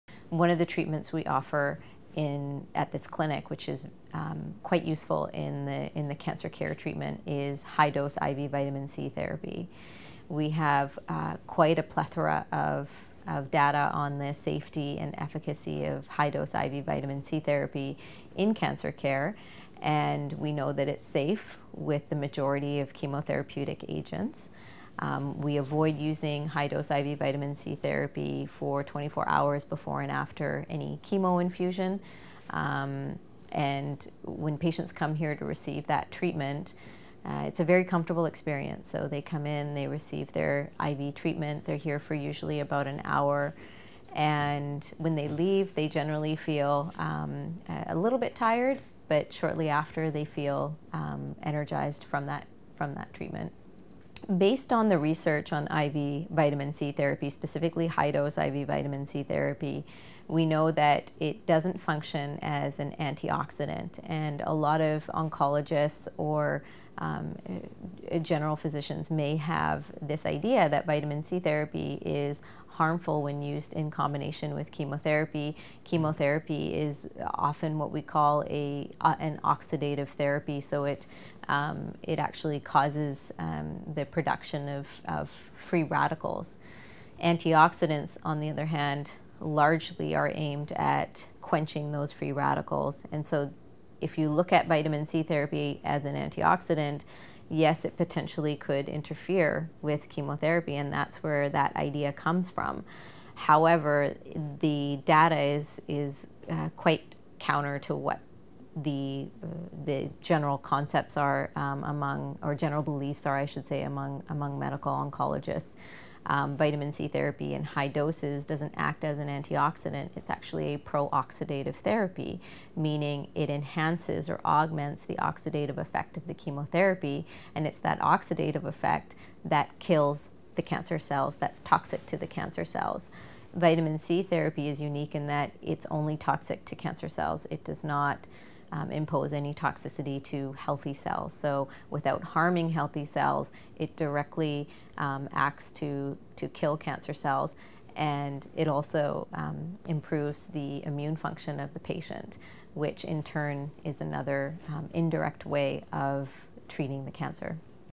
Port Moody Health High Dose Vitamin C Therapy
What is High Dose IV Vitamin C Therapy and how it is used in Cancer care?